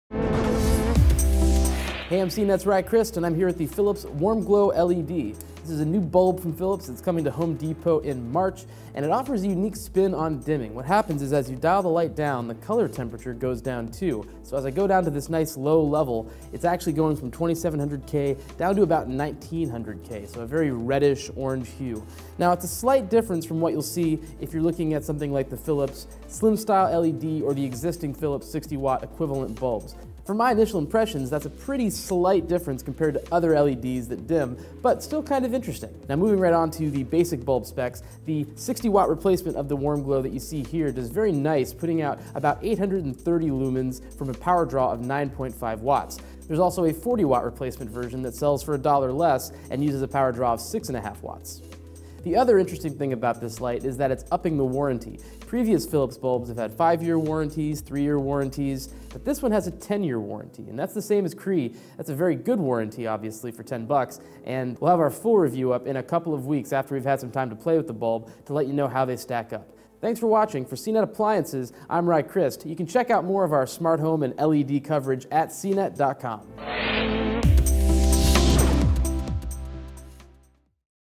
Philips' newest light bulb offers an LED sunset
The Philips Warm Glow LED drops the color temperature as you dim the light down low.